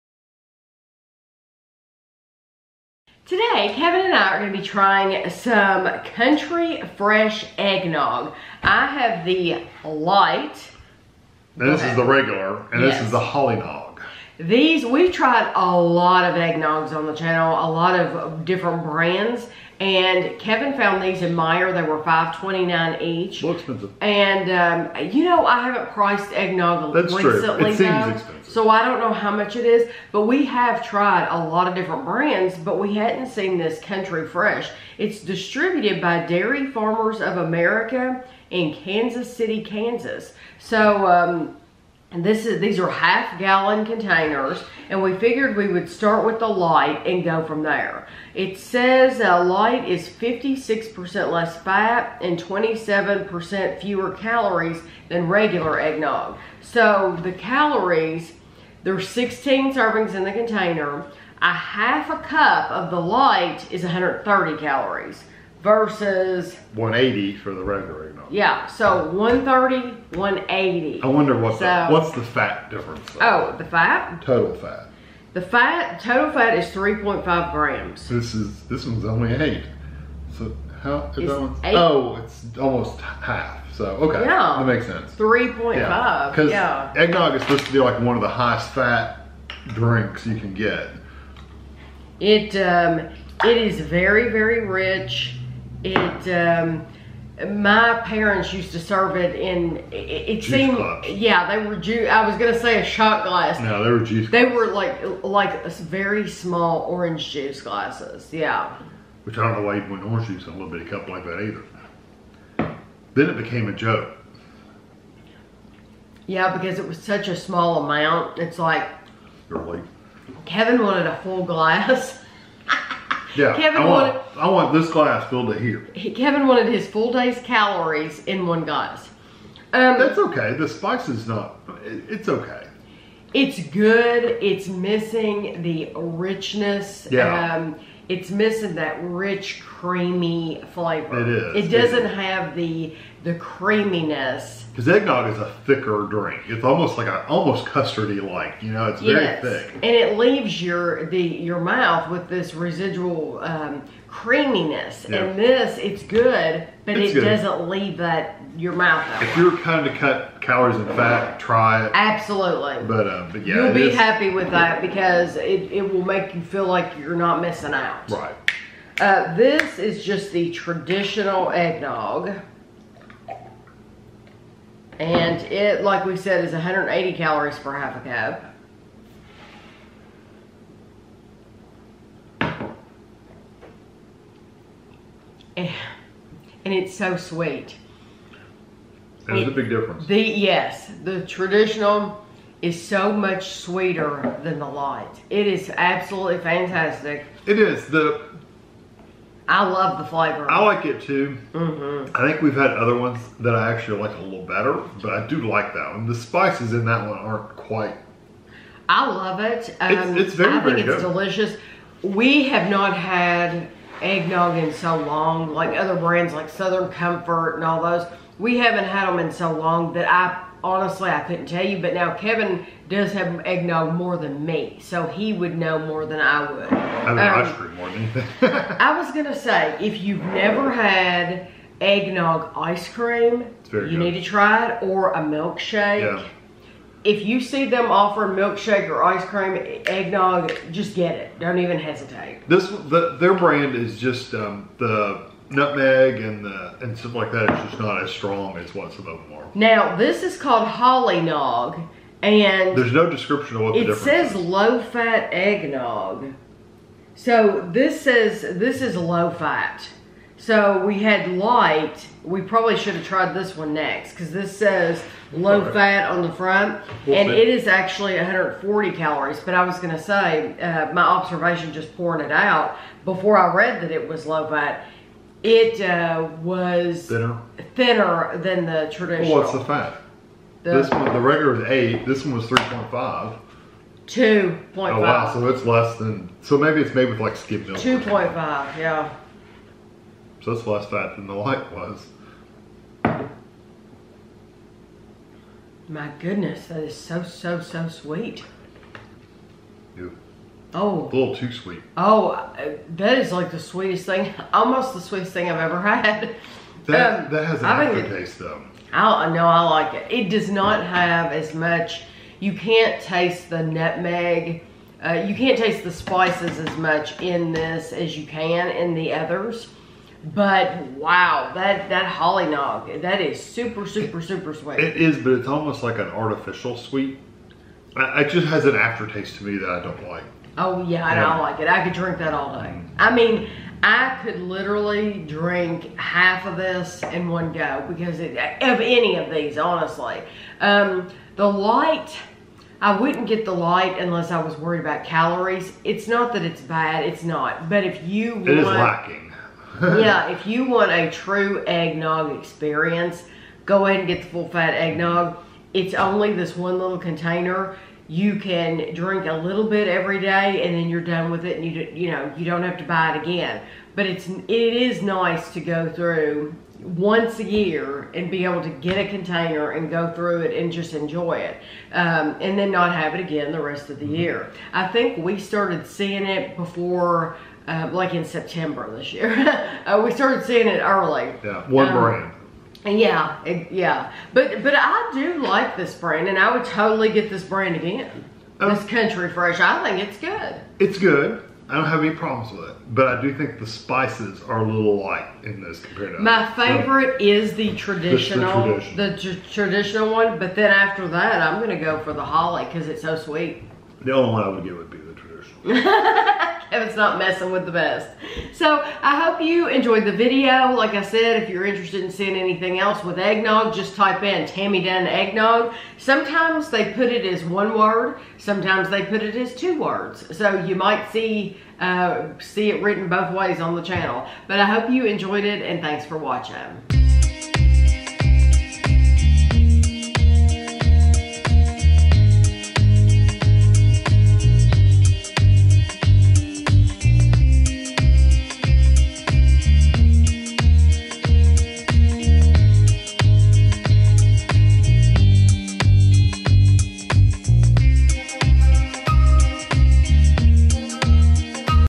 Country Fresh Eggnog: Lite, Original & Holly Nog Review
This is a taste test/review of Country Fresh Eggnog in three varieties including Lite, Original and Holly Nog. They were $5.29 each at Meijer.
* Lite ½ cup (120mL) = 130 calories
* Original ½ cup (120mL) = 180 calories
* Holly Nog ½ cup (120mL) = 140 calories